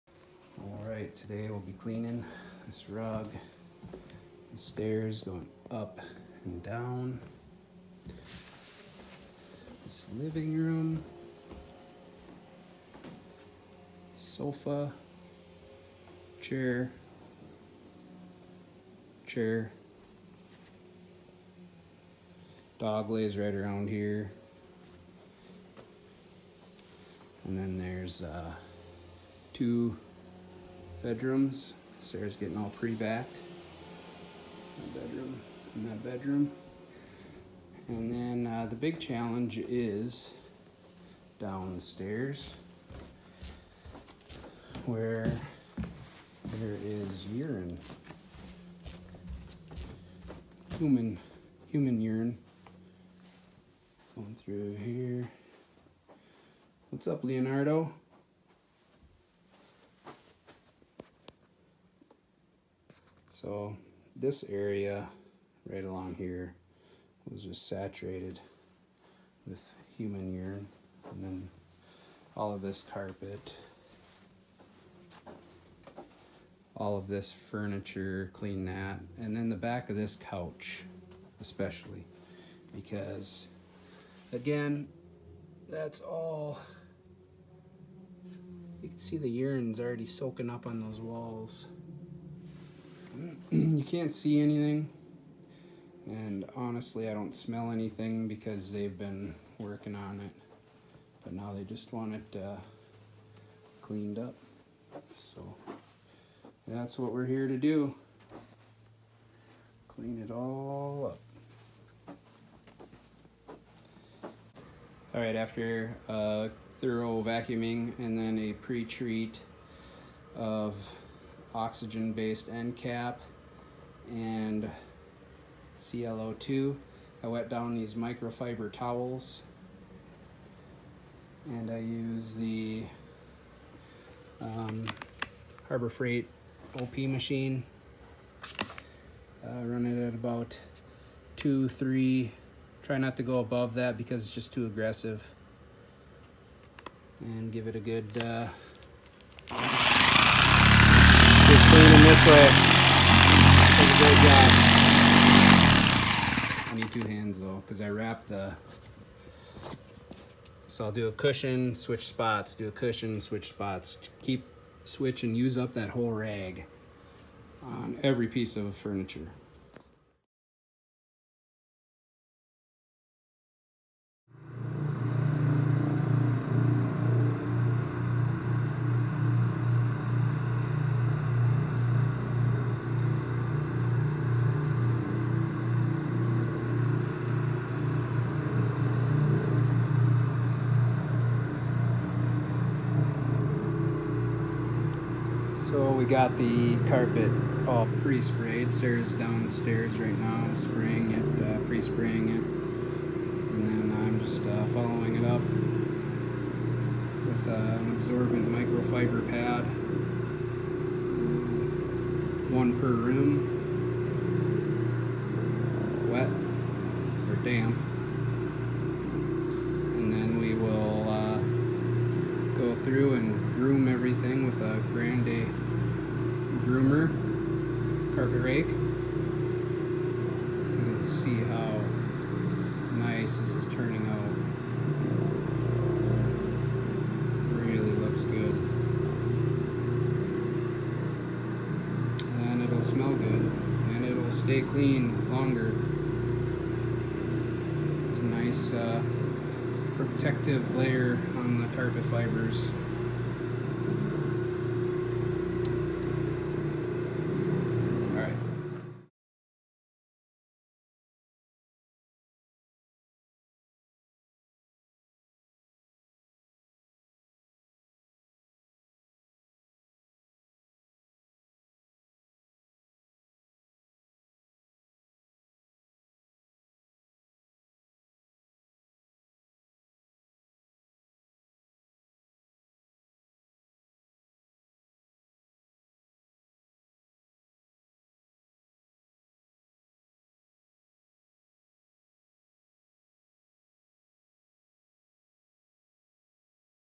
Low Moisture Cleaning sanitize, deodorize, and disinfect with cl02.
Low moisture cleaning residential to take care of some urine and give it a freshen up.  The cl02 kills the urine once it makes contact with it.  So we prespray all the spots with first with cl02.